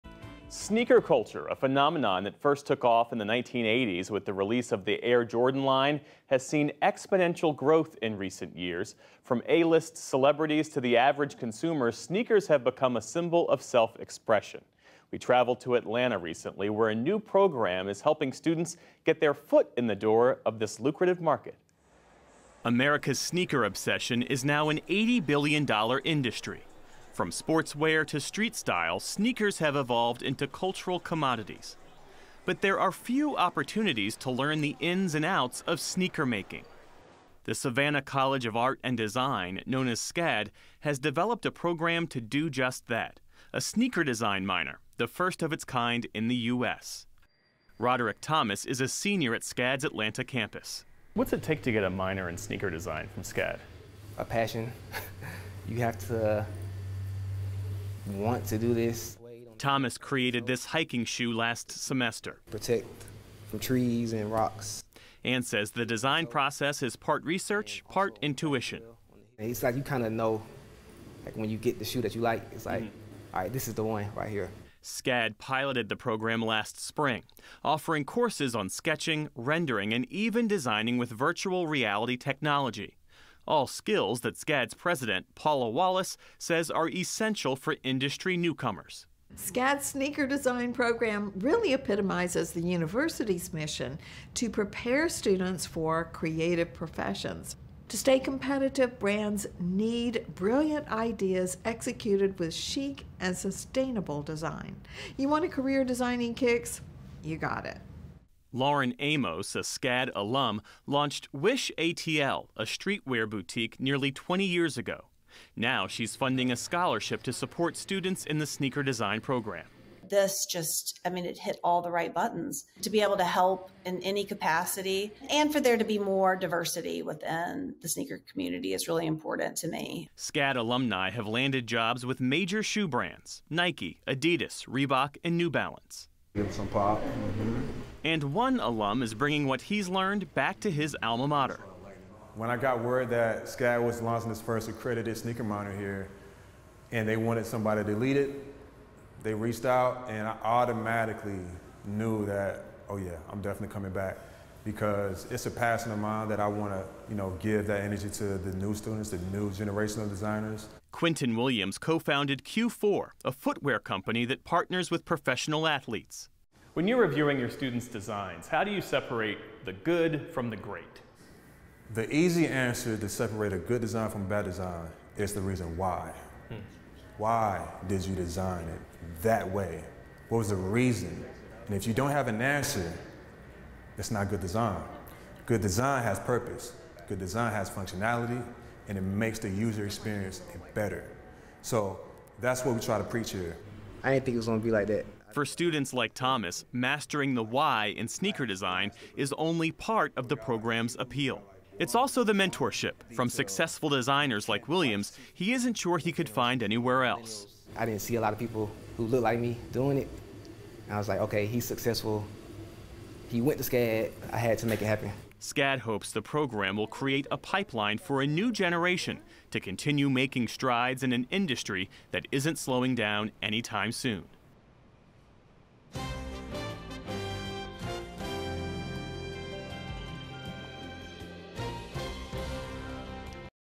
Why these students in Atlanta are studying sneaker design
From A-list celebrities to the average consumer, sneakers have become a symbol of self-expression and culture. In Atlanta, a new program at the Savannah College of Art and Design is helping students get their foot in the door of this lucrative, $80 billion industry. Geoff Bennett reports.